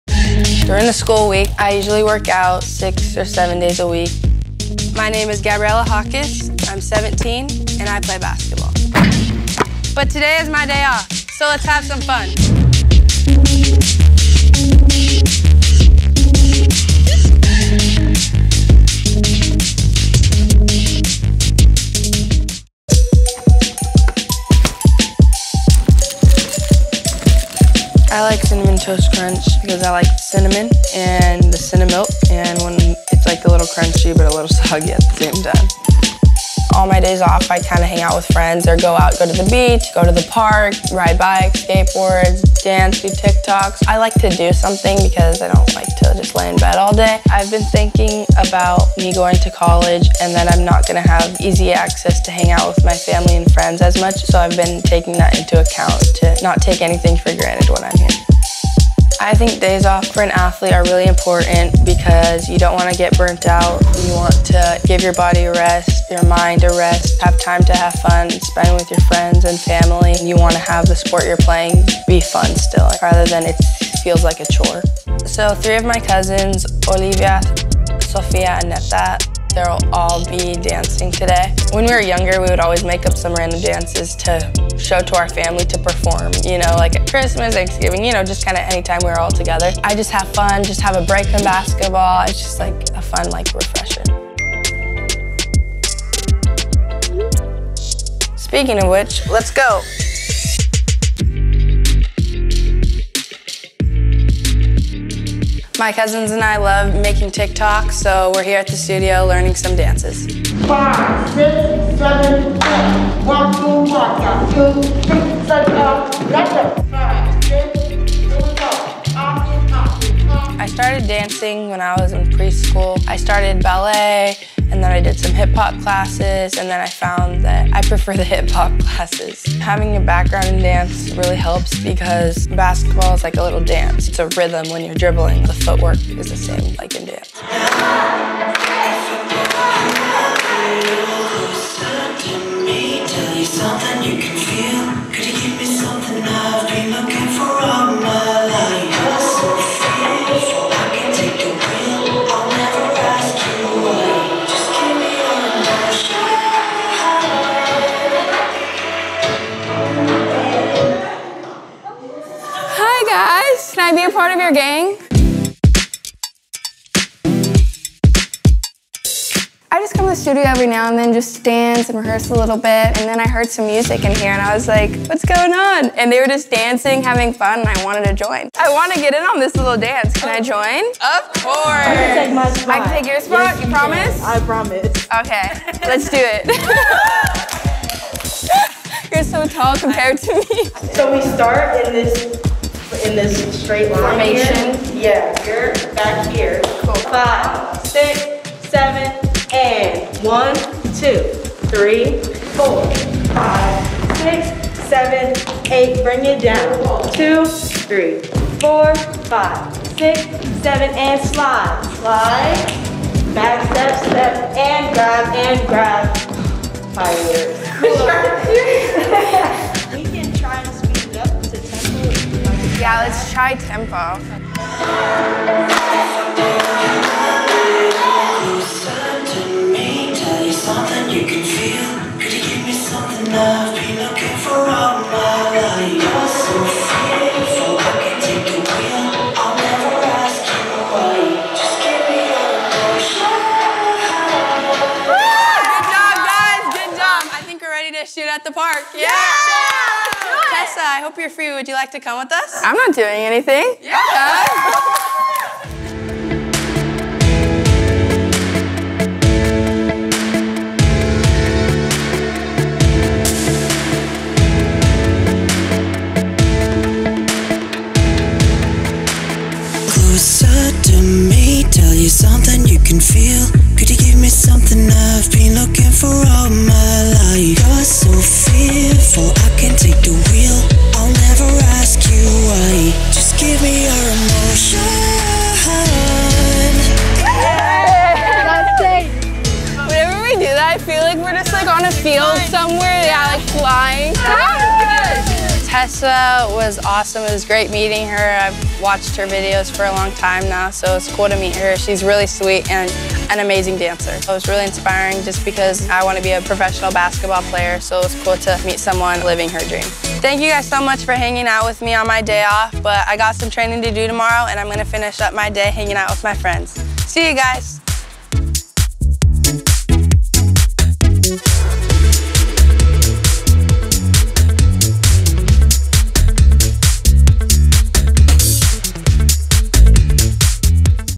17-Year-Old Hoops Star Gets A SURPRISE Visit From Tessa Brooks! 👀
17-year-old hooper, Gabriela Jaquez takes some time away from her training to meet up with social media star, Tessa Brooks, and show she's got moves both on and off the court... and maybe even go viral on TikTok!